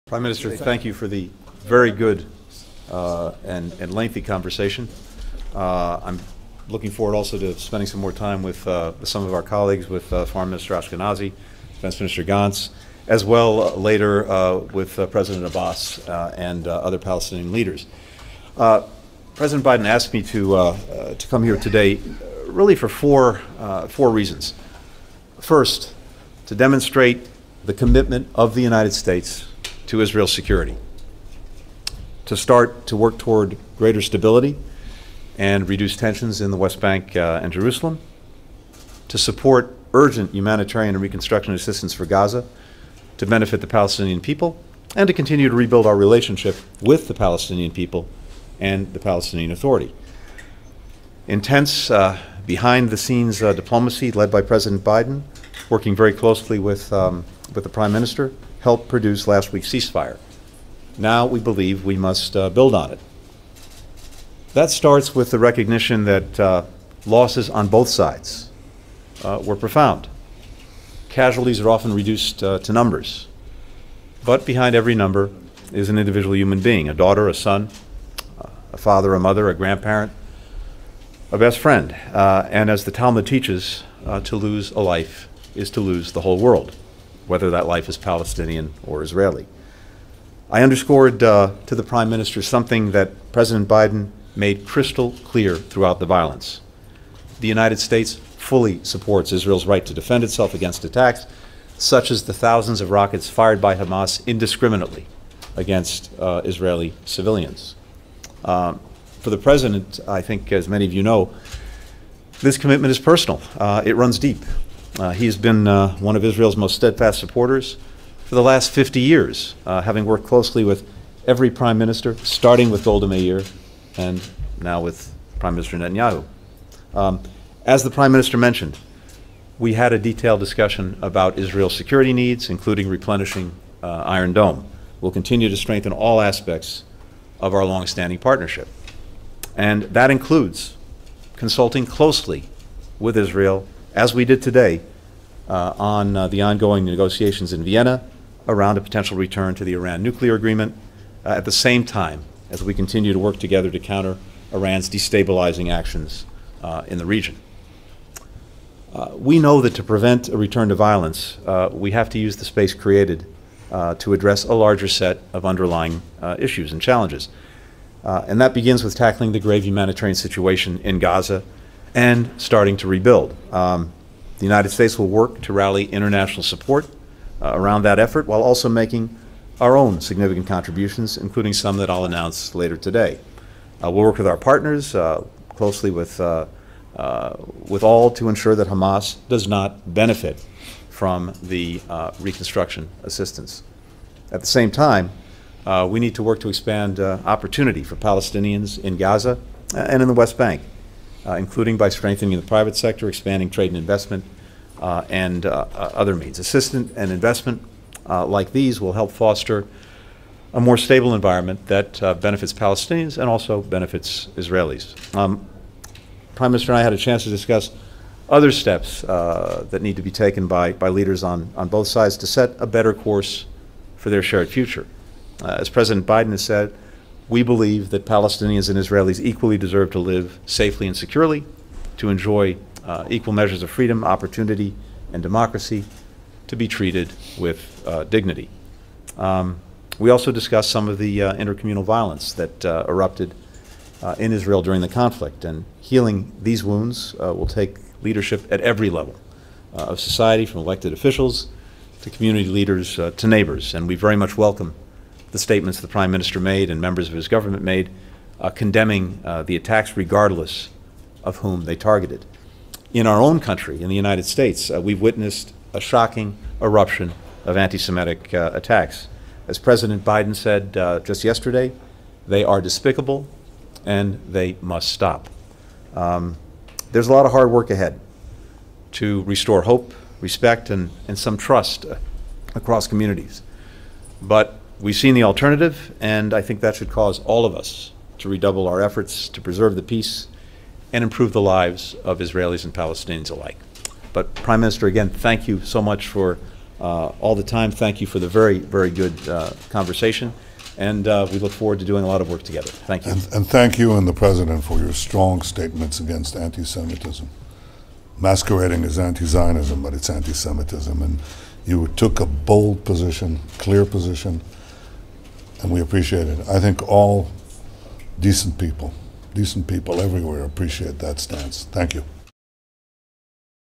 Tony Blinken | Statele Unite susțin pe deplin dreptul Israelului de a se apăra împotriva atacurilor
Tony Blinken | Statele Unite susțin pe deplin dreptul Israelului de a se apăra împotriva atacurilor la întâmplare cu mii de rachete ale Hamas împotriva civililor israelieni

CIF: 33686860